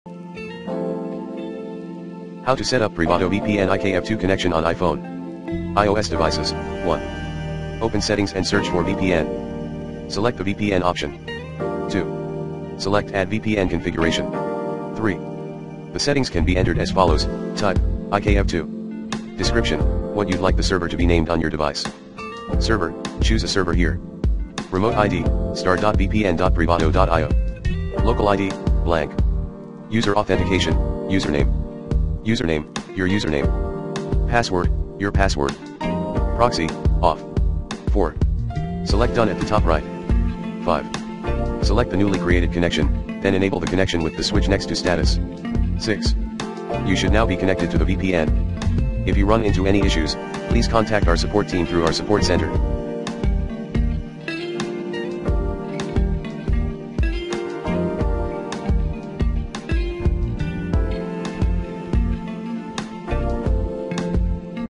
Set up PrivadoVPN IKEv2 Connection on iPhone/iPad
Here's how to Set up PrivadoVPN IKEv2 Connection on iPhone/iPad. Hit & set up PrivadoVPN on your device later.